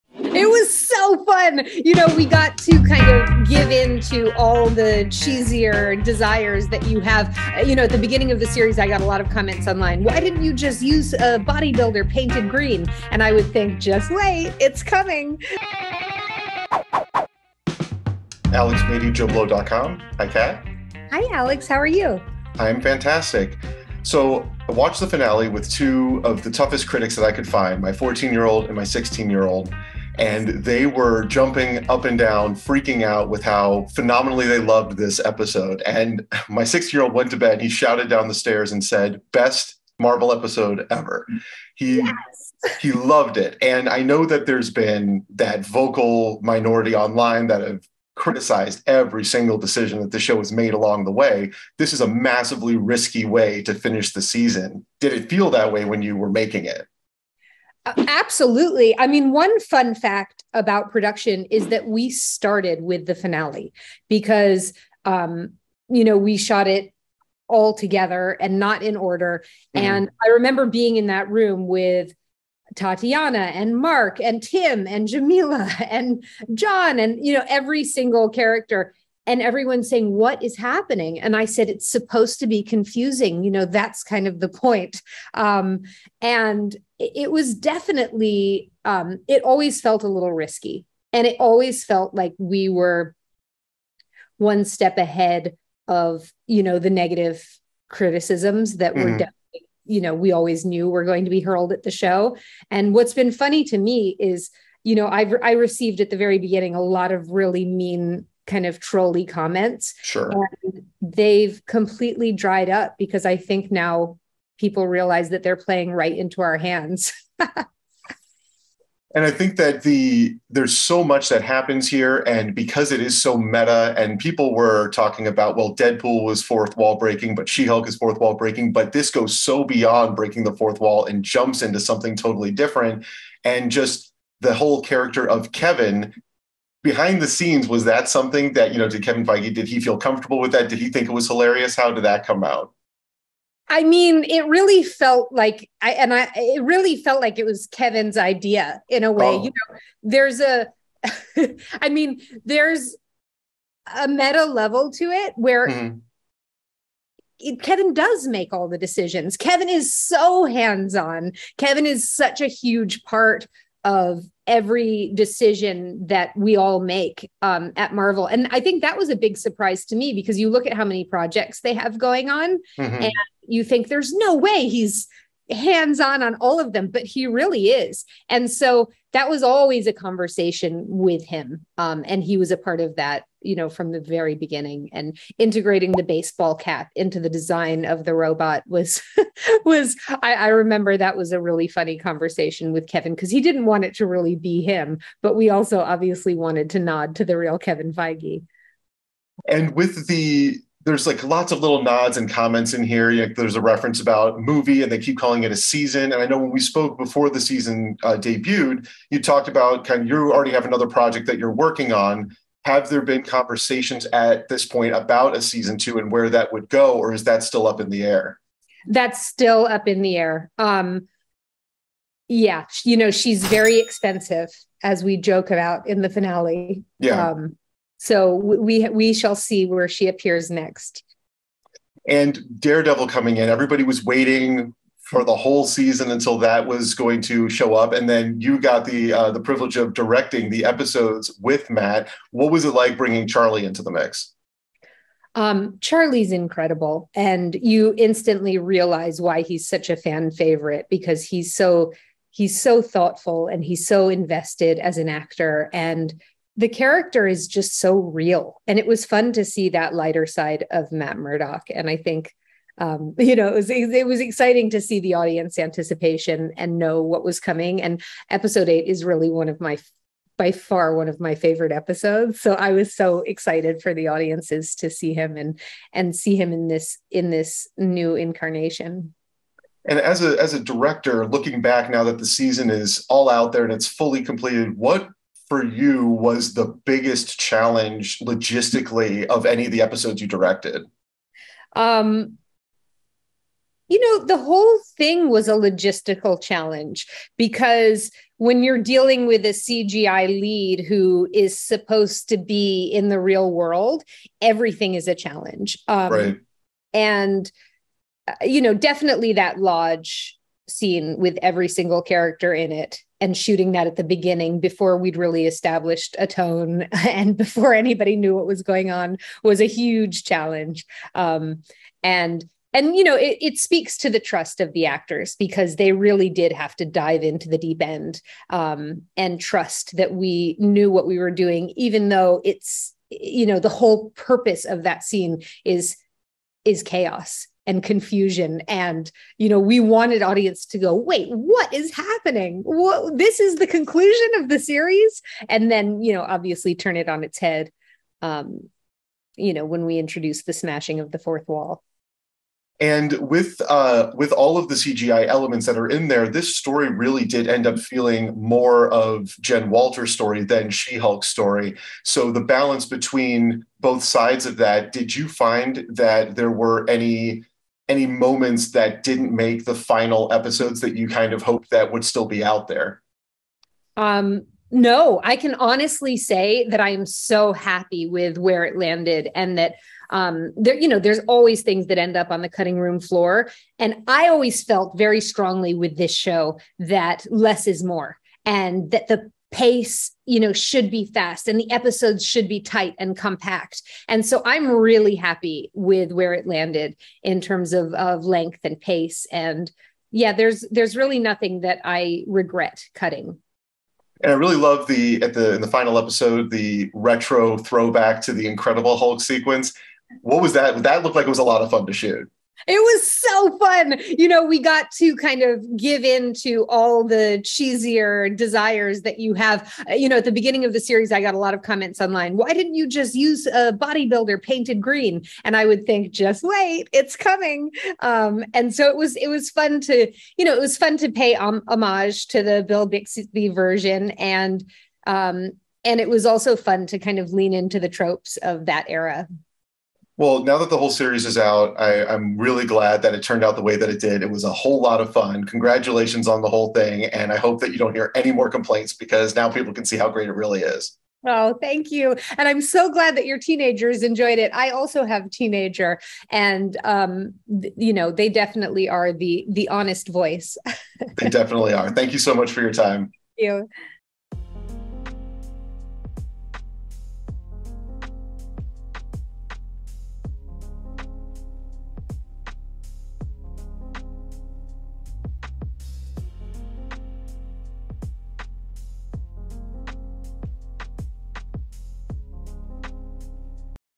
Interview: She-Hulk director Kat Coiro on trolls, Daredevil, Season 2 and more
JoBlo.com sits down with Kat Coiro the day after the season finale of She-Hulk and we talked about the trolls, the risks taken in the finale, the introduction of Daredevil to the MCU, and what may be in store for a second season…or if it is even official yet. Check out my full conversation with Kat Coiro in the embed above.